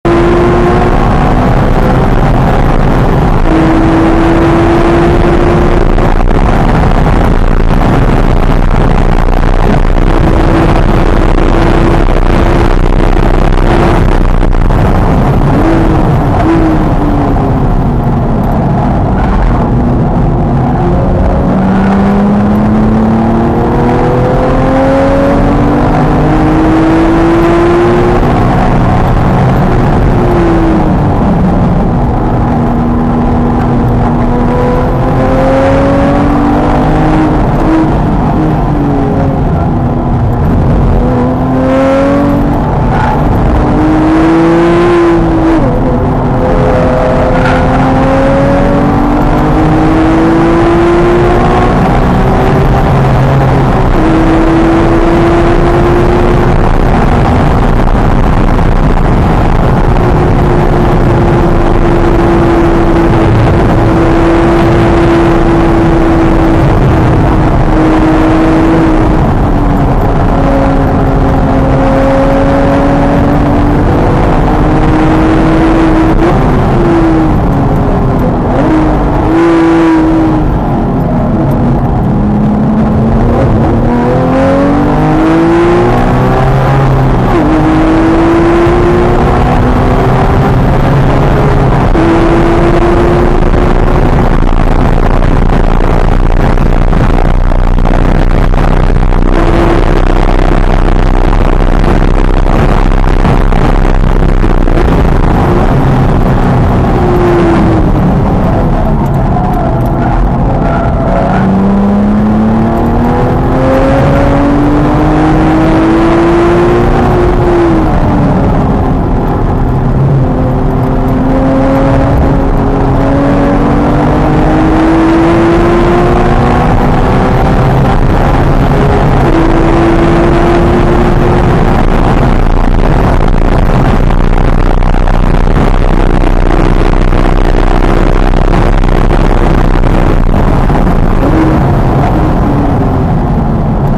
VIR 2:05 with passenger on Trofeo R tires in GT3
Series of new personal bests at VIR in the GT3. November 2012, perfect day 55 degrees and sunny.